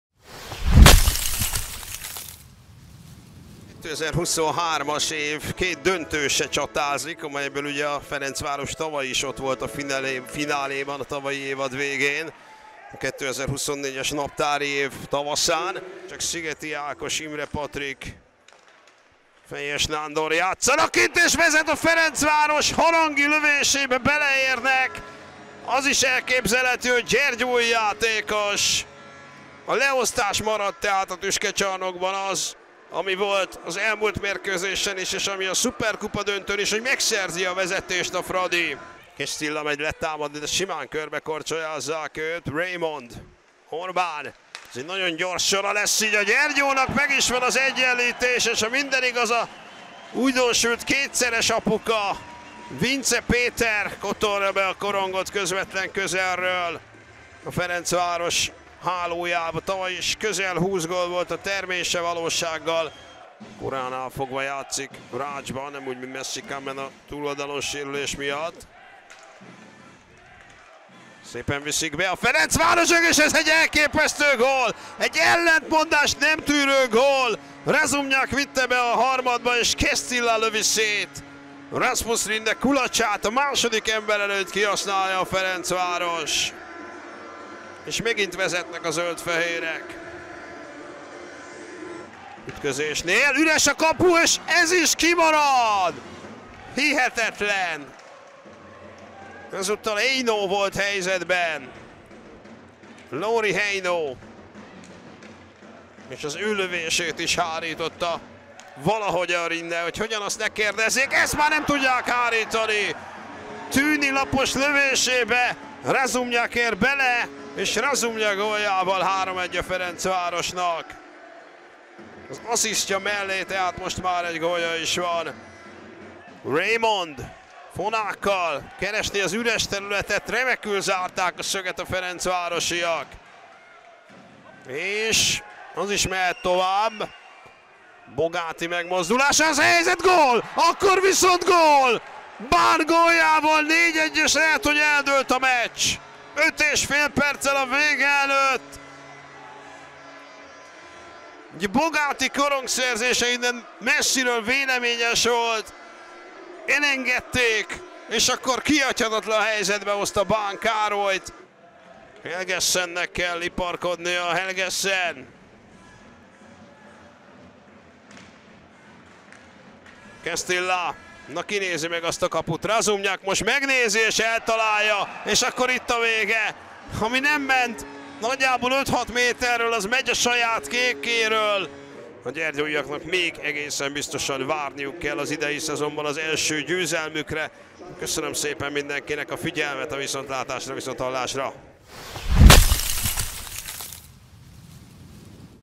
FTC-Telekom - Gyergyói Hoki Klub 5-1 (összefoglaló)
Erste Liga 2024/25, alapszakasz, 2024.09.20.